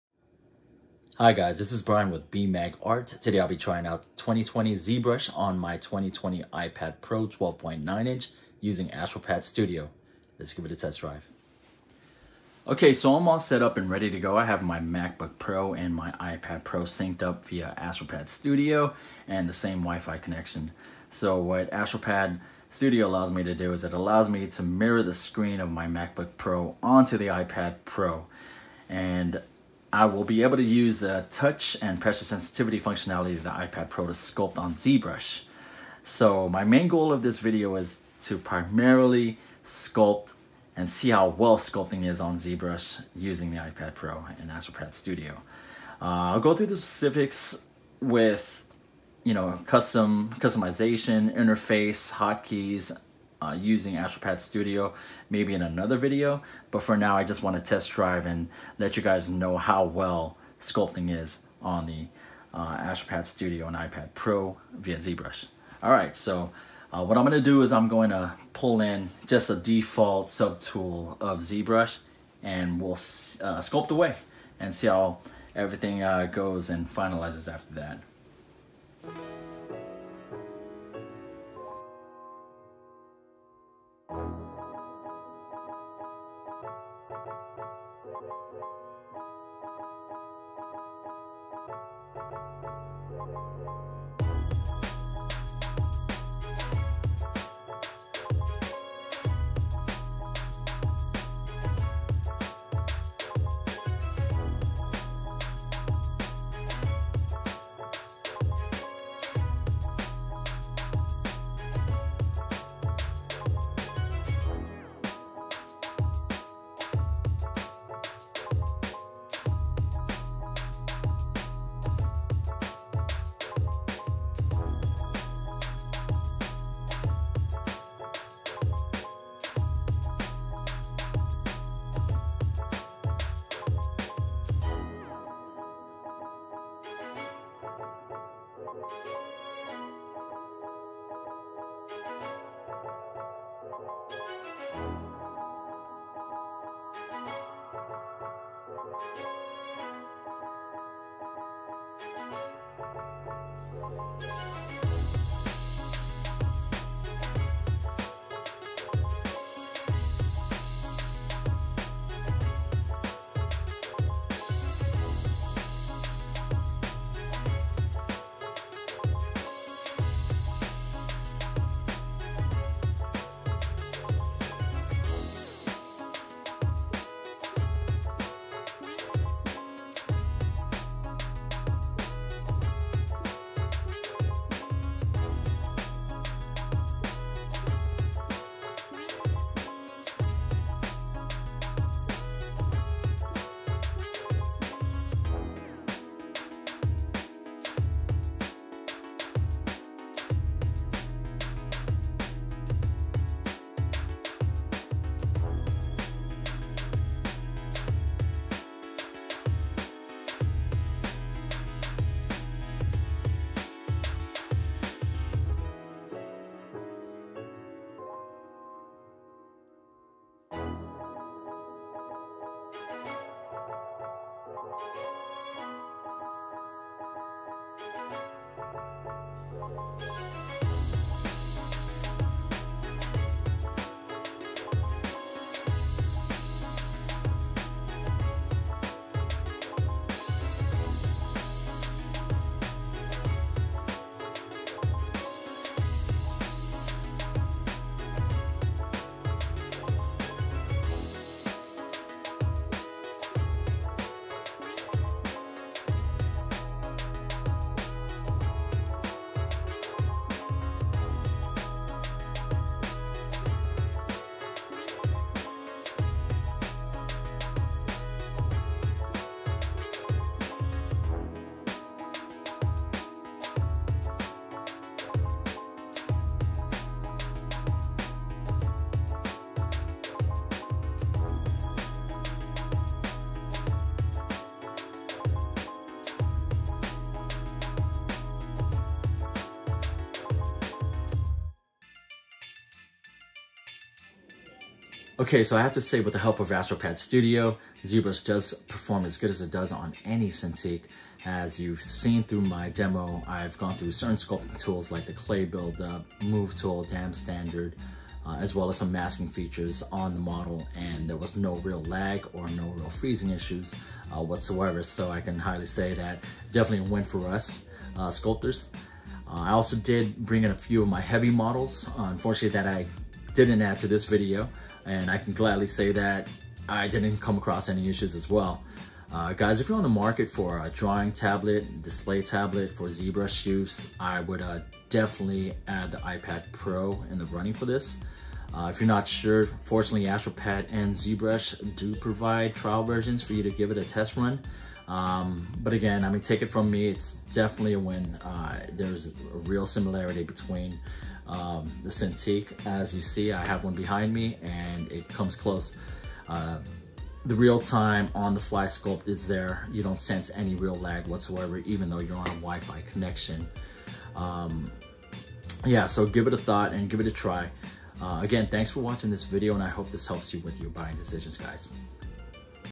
Trying Zbrush 2020 on the 2020 iPad Pro 12.9 Inch
A quick video of using ZBrush 2020 on the 2020 iPad Pro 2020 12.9 inch.